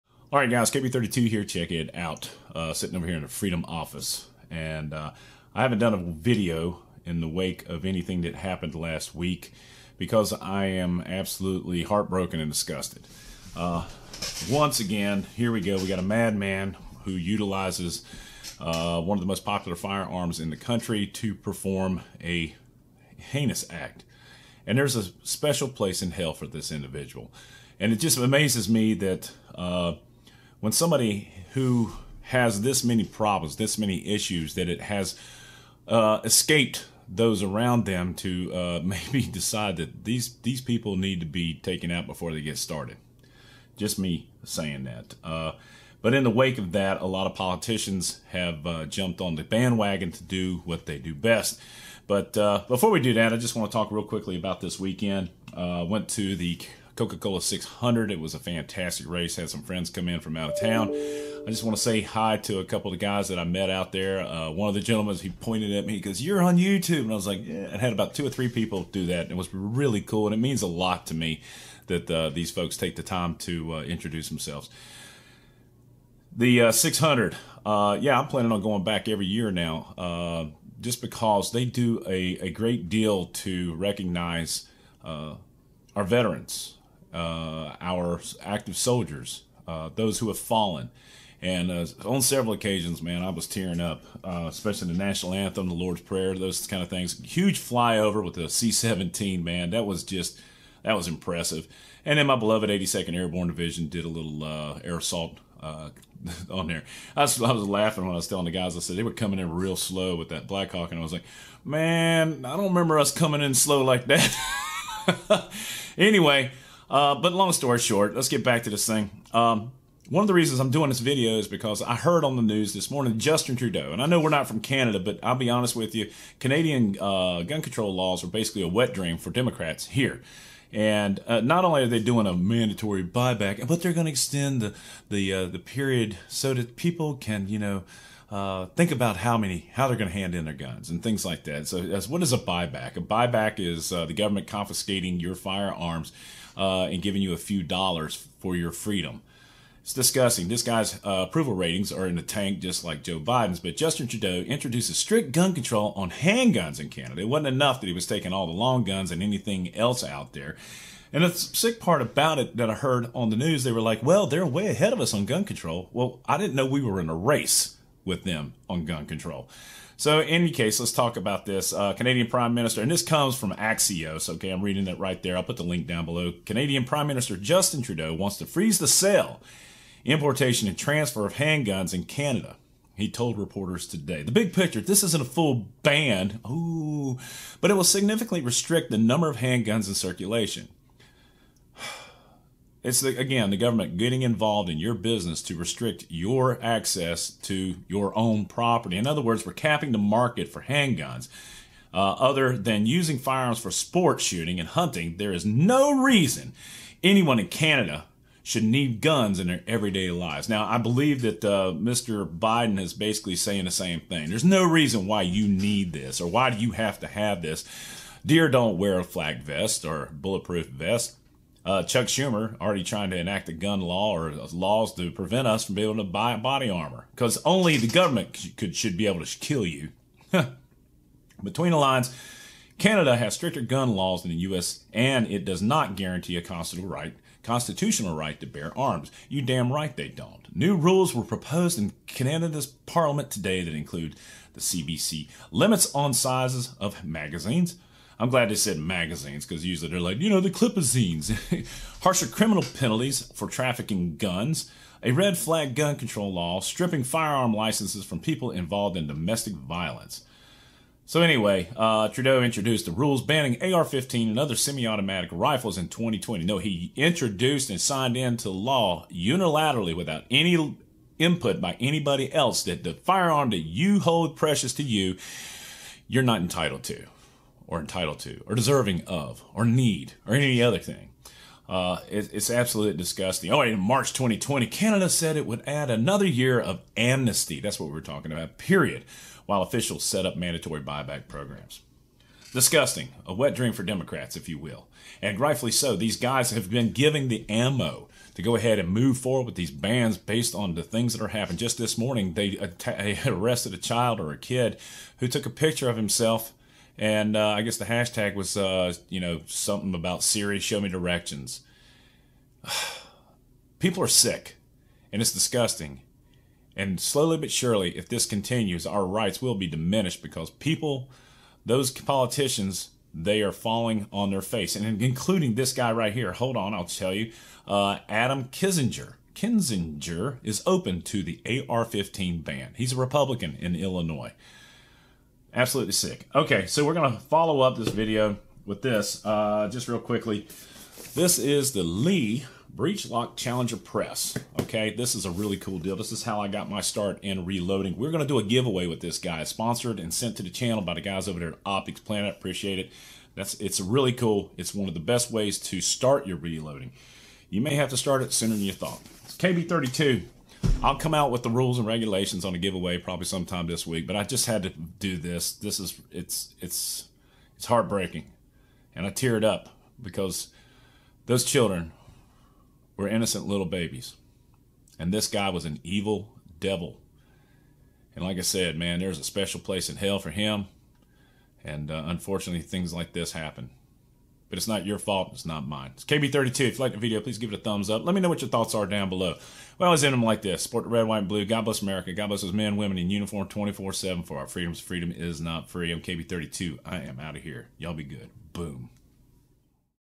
Canadian Gun Control a Dream for US Democrats!
In the wake of a tragic event, many people are taking this opportunity to expand their views on more gun control.  This is a generational fight that will never end.  As long as there are bad people in our world, bad things will continue to happen.